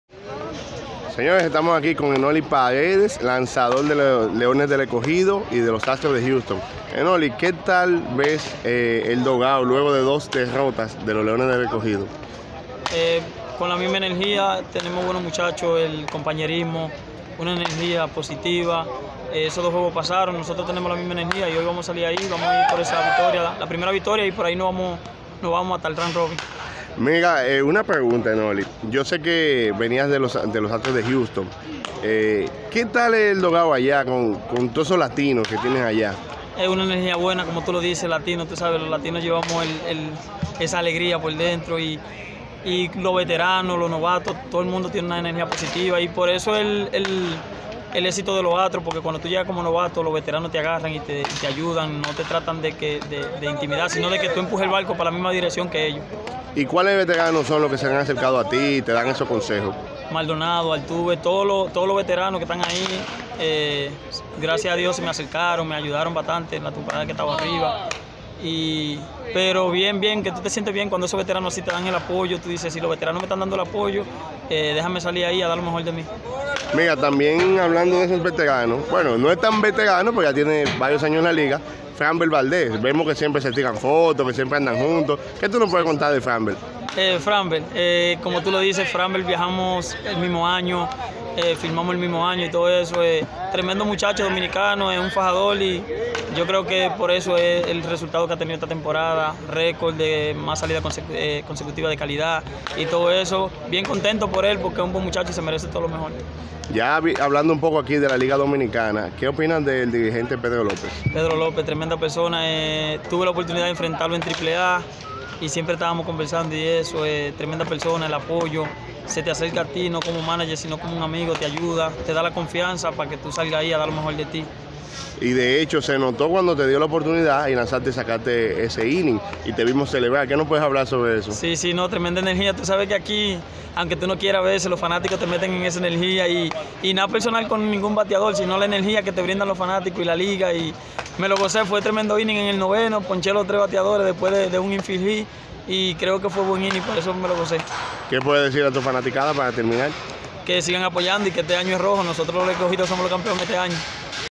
Enoli Paredes ``Franmber Valdez es un fajador por eso es el resultado que ha tenido esta temporada´´
Canal diseñado para el mundo del deporte con la finalidad de darles el mejor contenido del mundo desde la República Dominicana.